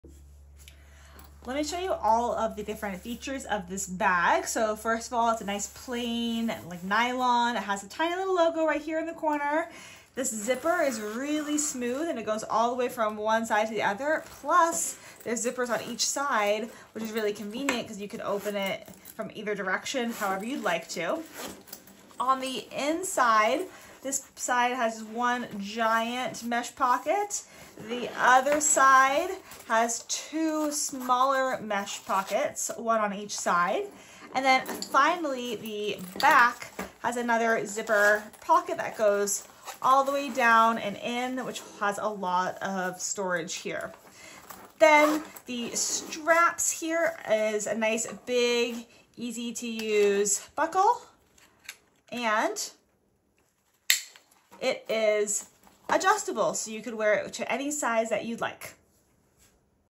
Close up details of this Pander Belt Bag
This small bag has multiple pockets and thoughtful design.